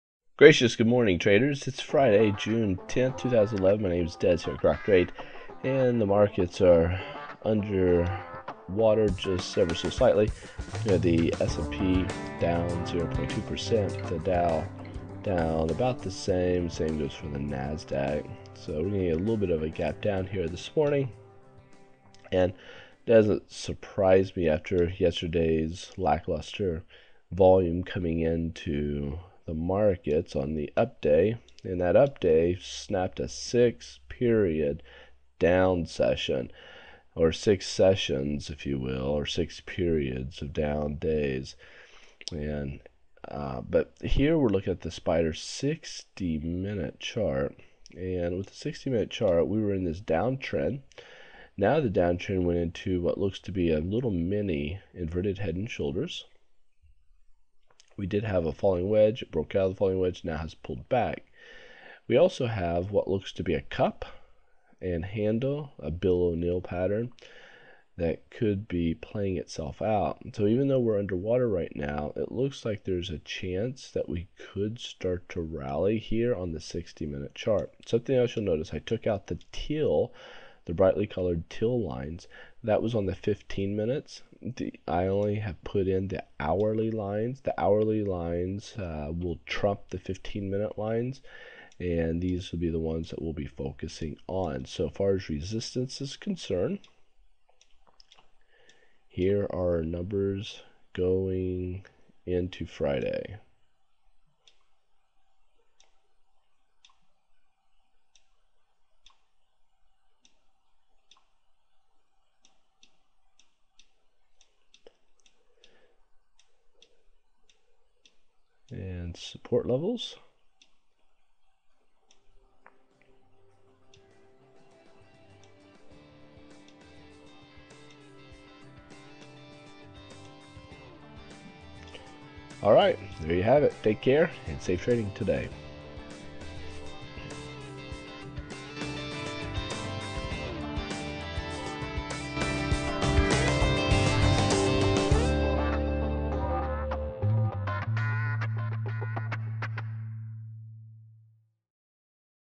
SPY Pre-Market Analysis June 10, 11
Pre-market stock futures mostly down under continued pressure.